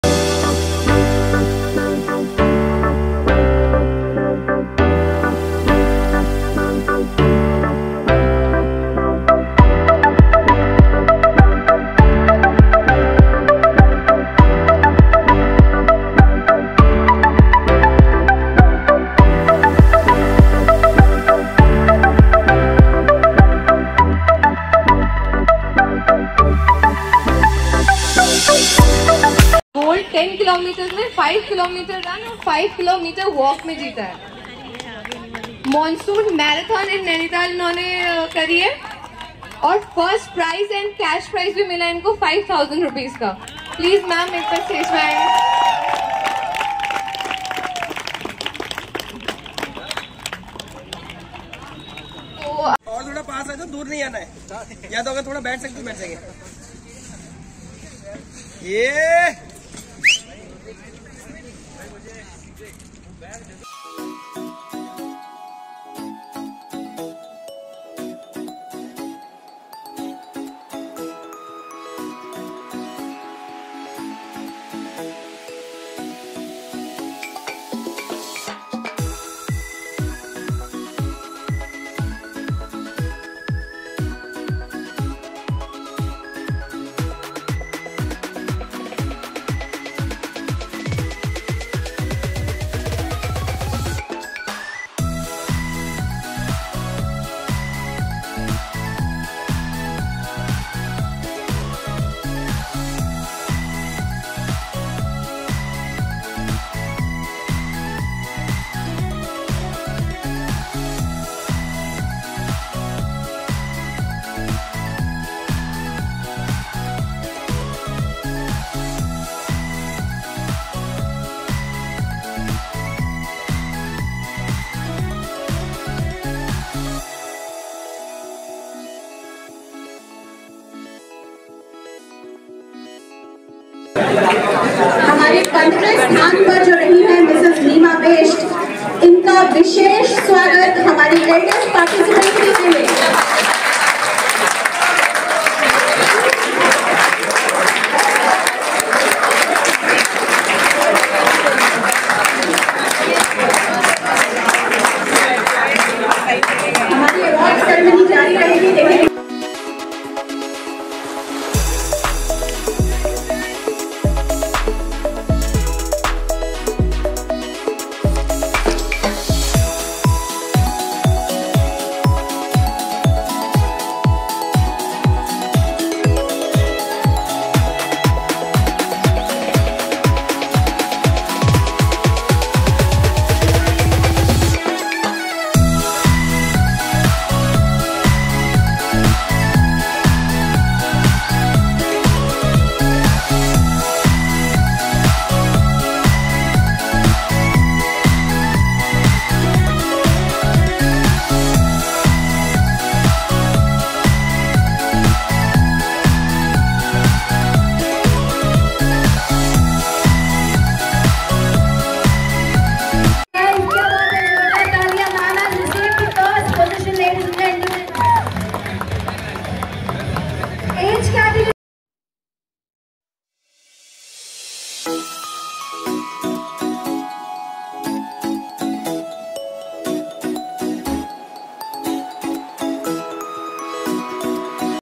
ACHIEVEMENT IN MARATHON#GIRISH BISHT
NEEMA IS 61 YEARS OLD.SHE STARTED RUNNING AGE 58. HER ACIEVEMENT OF 2023.